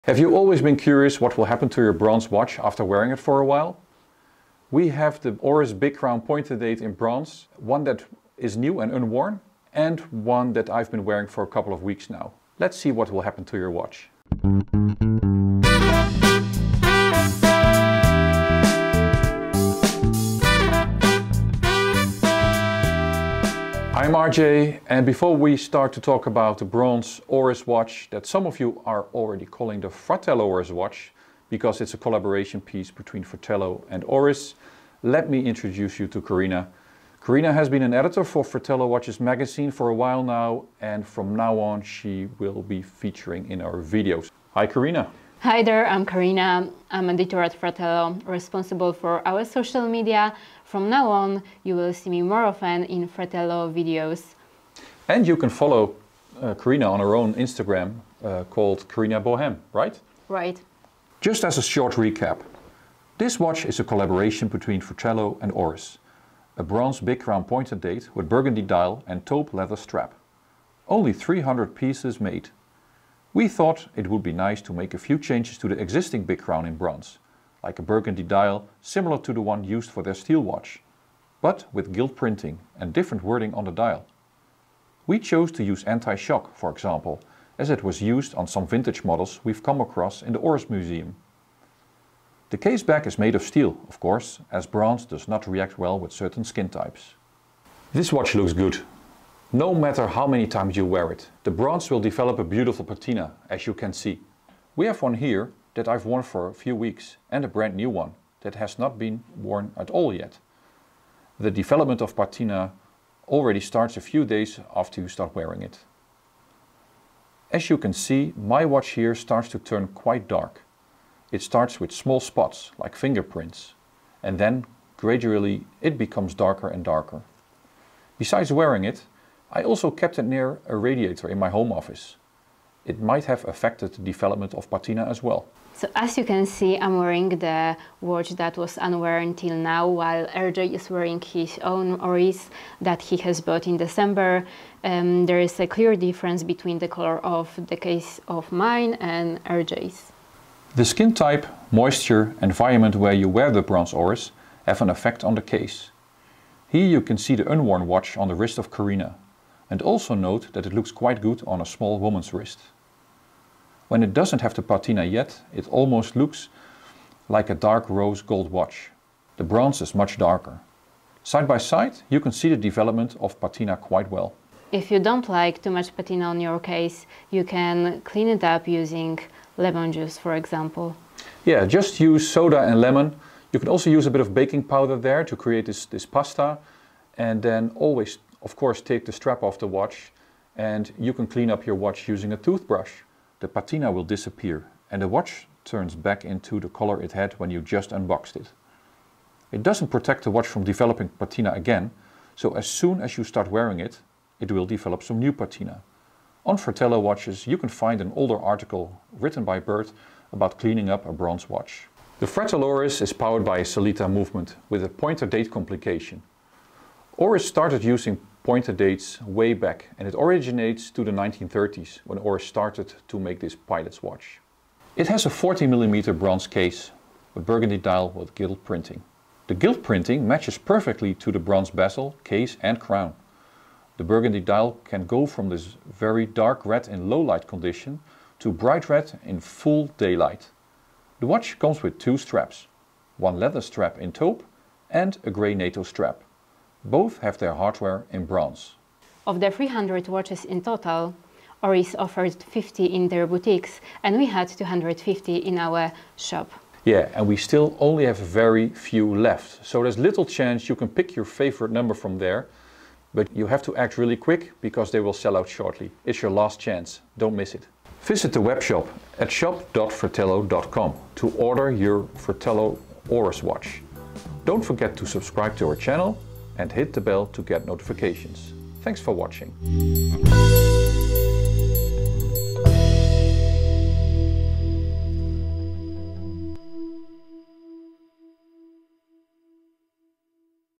Patina on Bronze Watches: a side-by-side comparison
Karina and RJ are showing you the difference between a fresh Oris Big Crown Pointer Date Bronze and one that has been worn for a number of weeks. How quickly will it start to develop a patina, and how can you get rid of it? Enjoy our video!

We are almost out of stock with this Oris, act quickly if you still want one

Content of this video:

00:00 - Introduction
01:01 - Short Recap Fratello x Oris Collaboration
01:37 - Side-By-Side Comparison
02:55 - Don’t Like Patina? Try This!
03:32 - Fratello x Oris Watch Specification